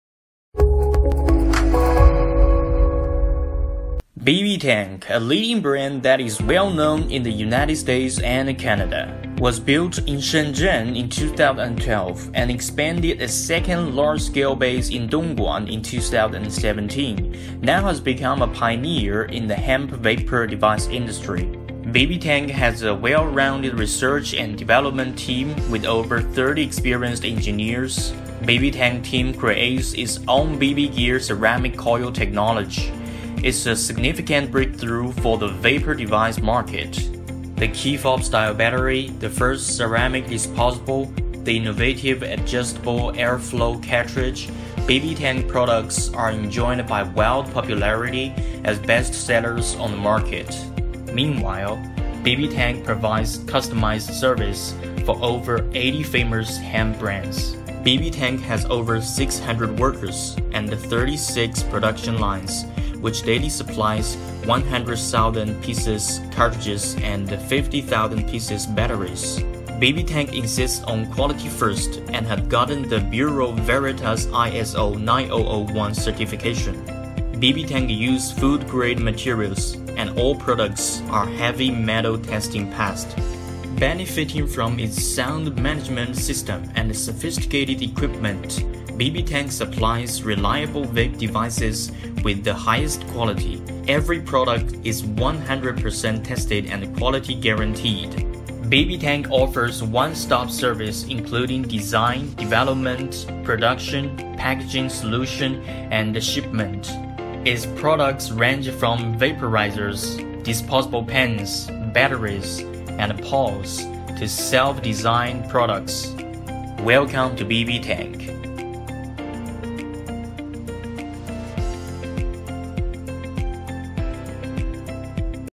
BBTANK’s goal is to help you achieve Yours.
Introduction of BBTANK. Our job is to avid vape company in their pursuit of excellence.
From developing, producing, packing to shipping- we covers all your needs.